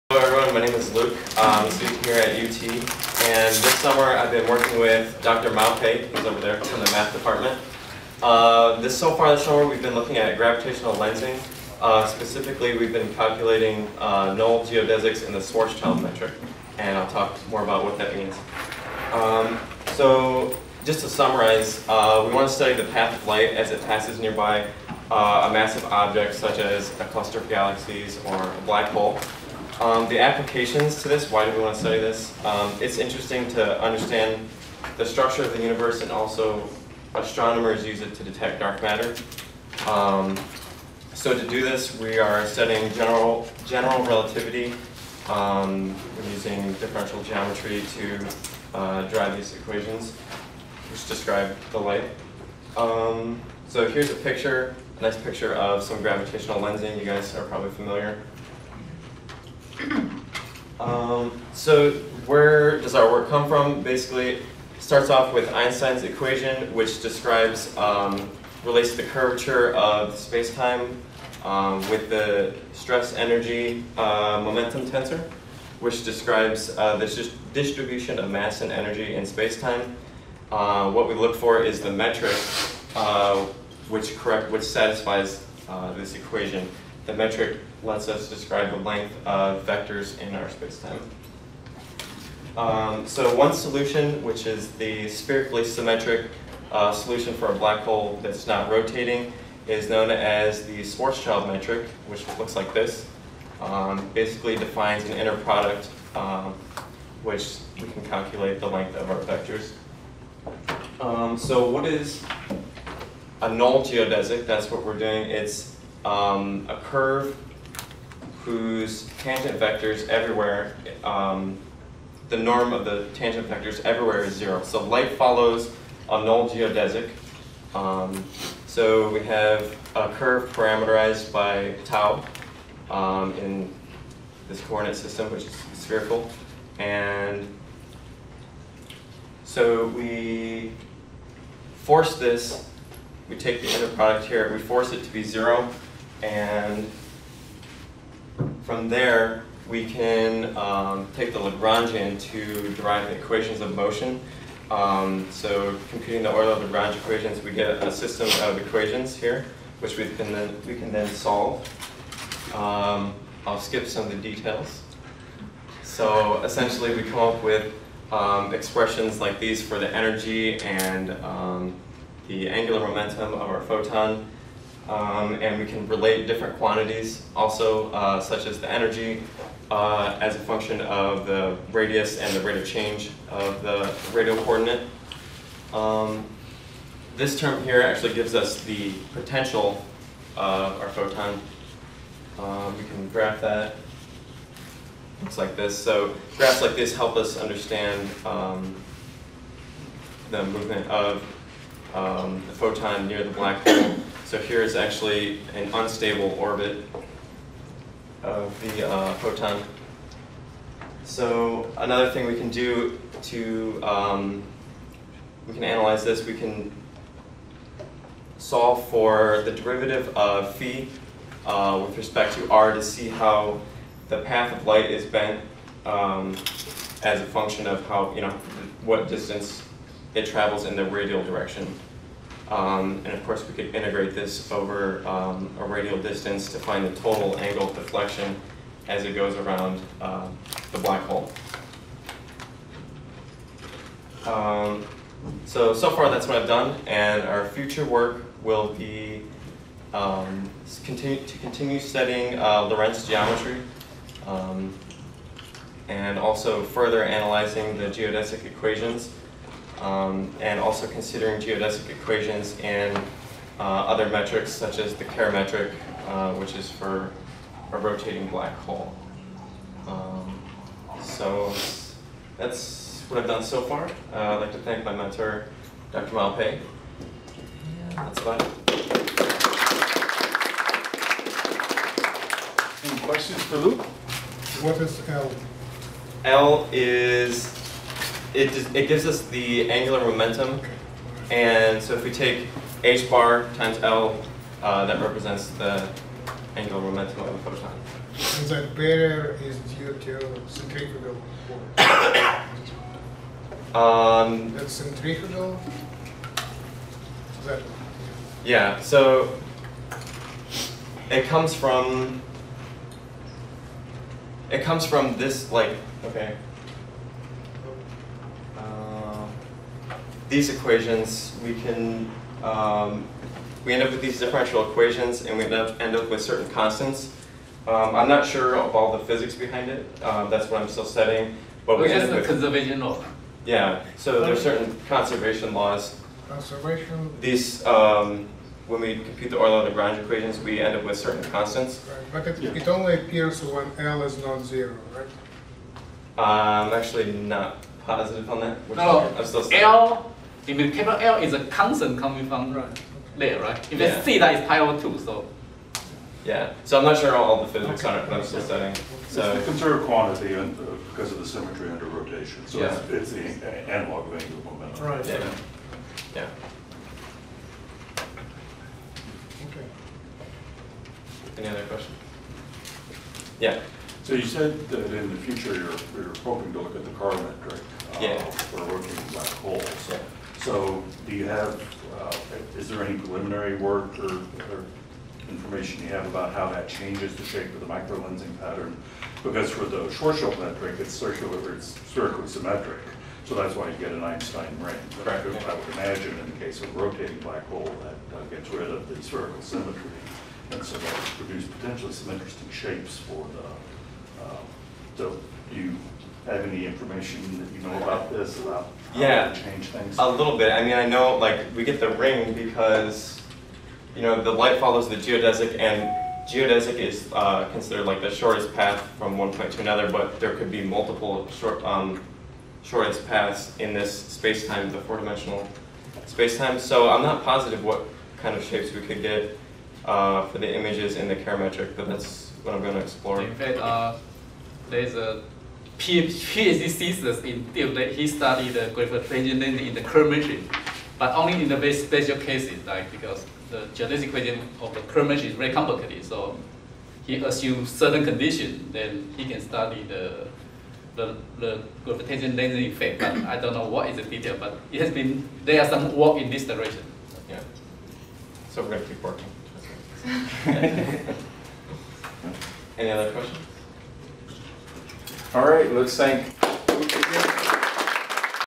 Luke Kwiatkowski - Calculations of Null Geodesics in the Schwarzschild Metric
University of Toledo Department of Physics and Astronomy 2013 Summer REU presentation by Luke Kwiatkowski.